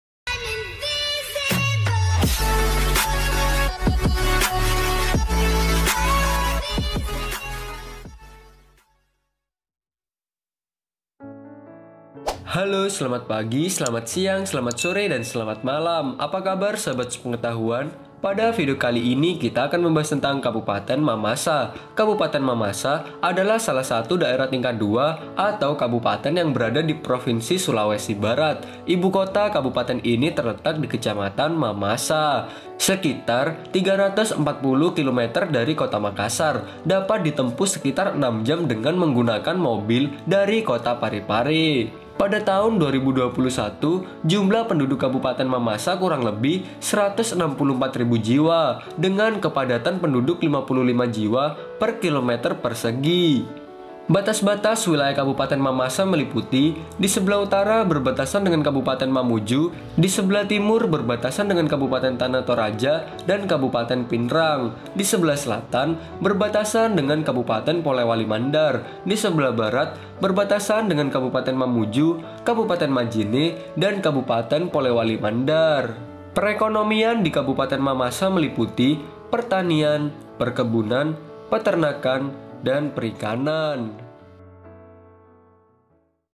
Kota Mamasa /Kabupaten Mamasa 2022 (Drone View) perbandingan infrastruktur dan skyline
halo guys.. saya di sini akan membahas tentang kemajuan kabupaten Mamasa provinsi Sulawesi Barat dan memberi info infrastruktur yang ada di kota ini
saran dan kritik: silahkan komen di bawah

Sumber : skyscrapercity Indonesia